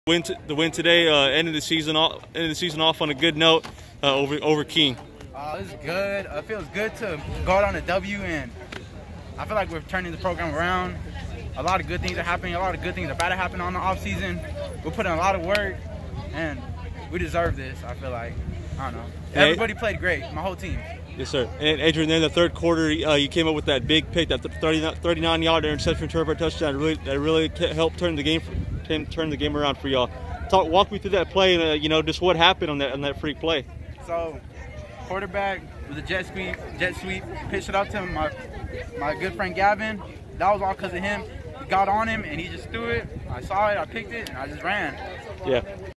Adrian Benavides on win over King and his INT return for a TD
Carroll Wraps Season on High Note